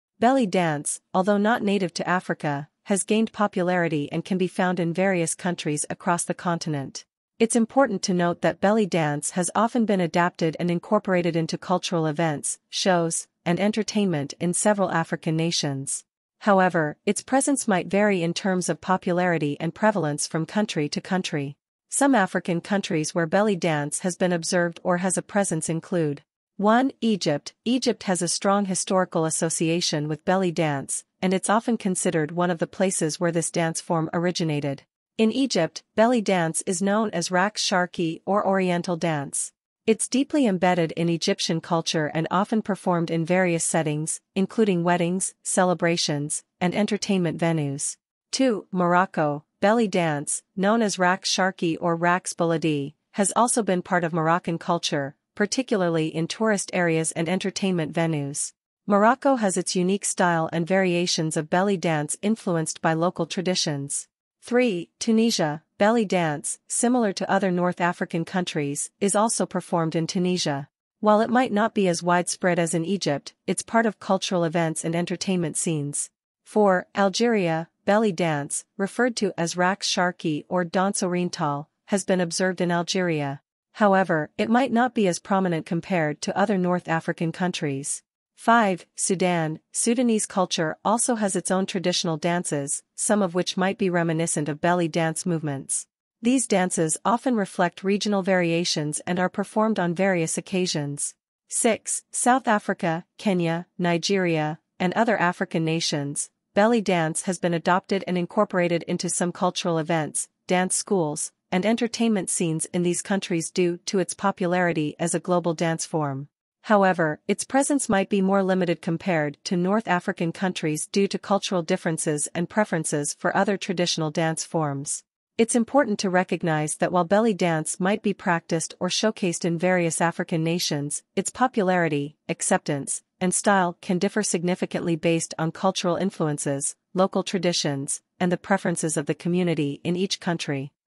The Rise of Belly Dance in Africa
Belly dance, originating outside Africa, has gained popularity across the continent, featuring in cultural events, shows, and entertainment in various African countries. It's known as "Raqs Sharqi" or Oriental dance in Egypt and is deeply ingrained in their culture, performed at weddings, celebrations, and entertainment venues. Similarly, in Morocco, referred to as "Raqs Sharqi" or "Raqs Baladi," belly dance holds significance, particularly in tourist spots and entertainment venues. Tunisia, Algeria, and Sudan also incorporate belly dance into cultural events, each with its unique names and regional variations. Moreover, countries like South Africa, Kenya, and Nigeria have also embraced belly dance, seen in cultural events, dance schools, and entertainment settings.